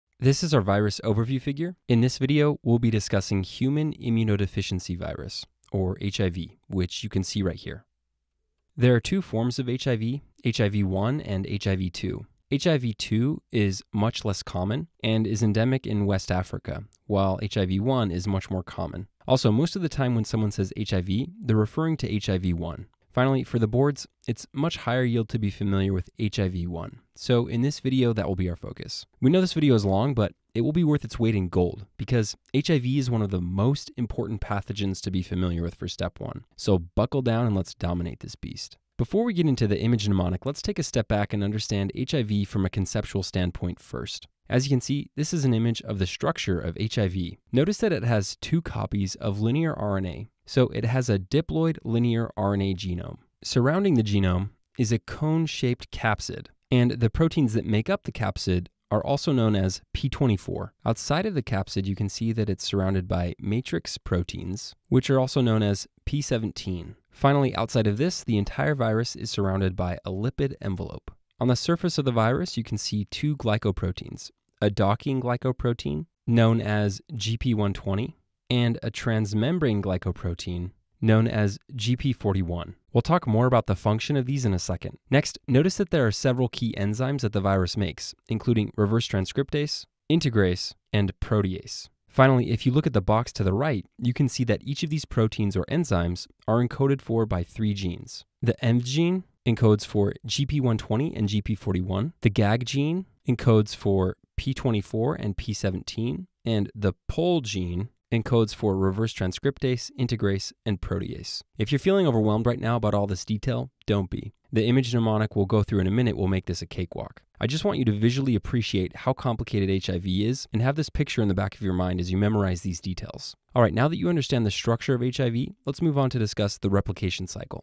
Human Immunodeficiency Virus — Know the virus behind immune collapse
If you’re learning how to pass USMLE Step 1, HIV is a must-know. From CD4 depletion to opportunistic infections, this virus connects immunology, virology, and high-yield pathology in one package.